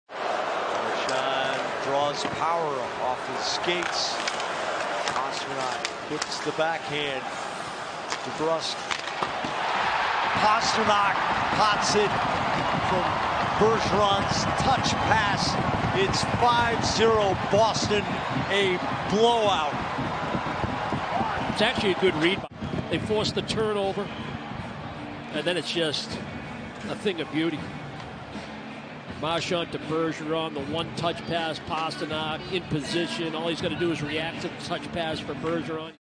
David Pastrnak scores vs Sabres (19 mar, 2023)
Pastrňákův 48.gól v sezóně proti Buffalu.